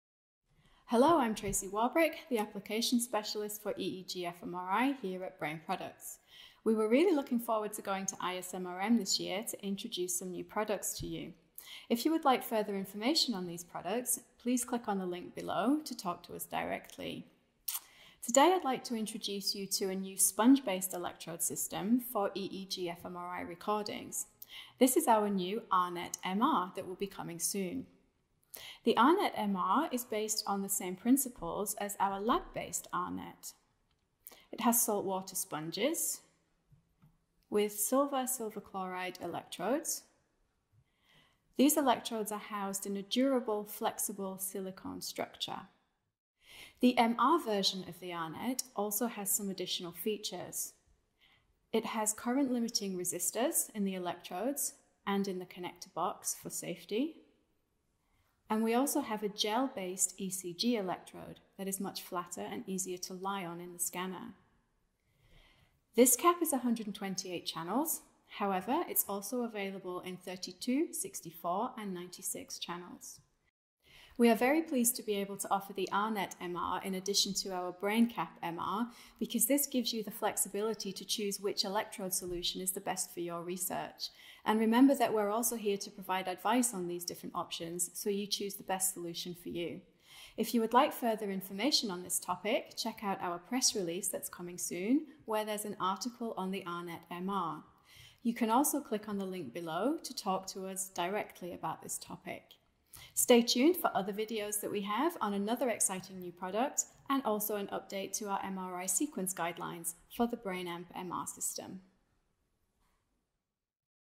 Introducing the R-Net MR
Get an introduction to our soon to be released sponge-based electrode system for EEG-fMRI recordings, the R-Net MR. Online events on this topic were held on April 21 and 22, 2020.